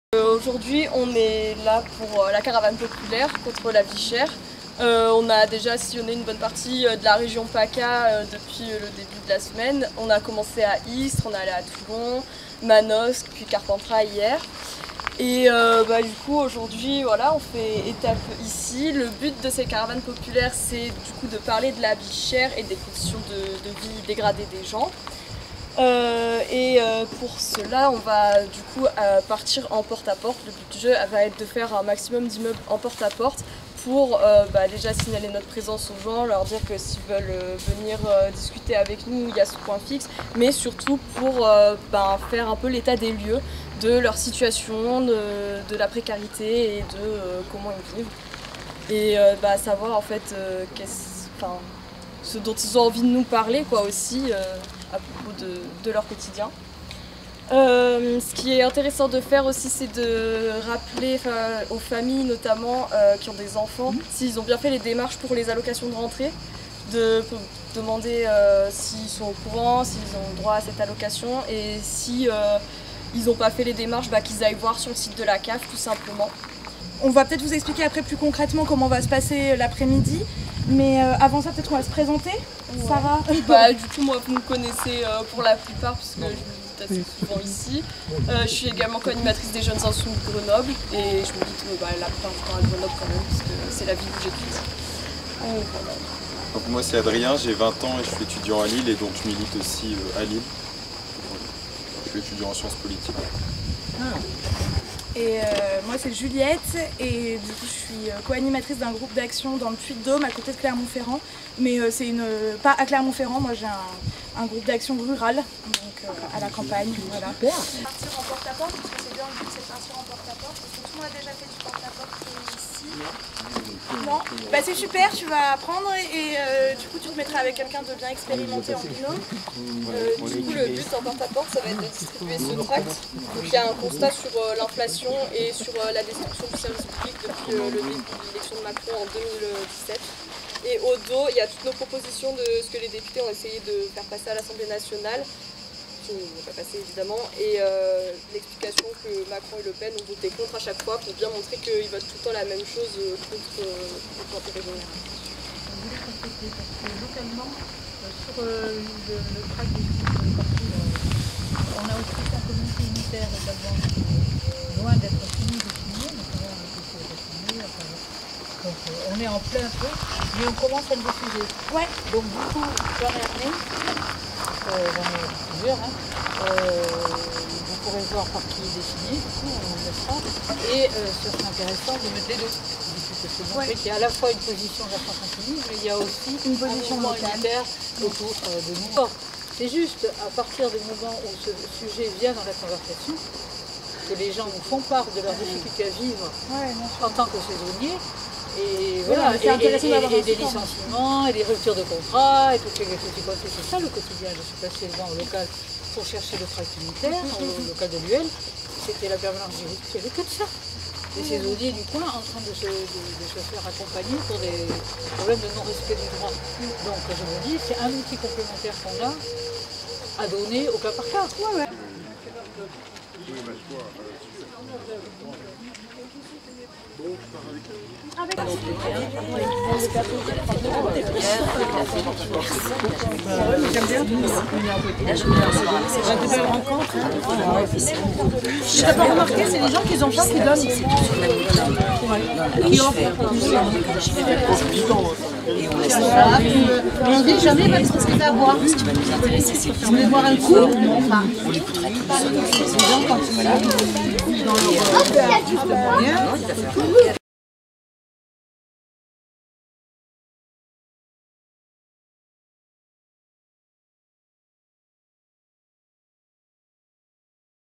devoirs de vacances...
qui fait quoi pendant les vacances pour lutter contre macron et le pen? ...la vie plus chère , les services publics sans finances qui se délitent , les milliardaires toujours plus avantagés? ...au fait qui vote quoi à l'assemblée nationale?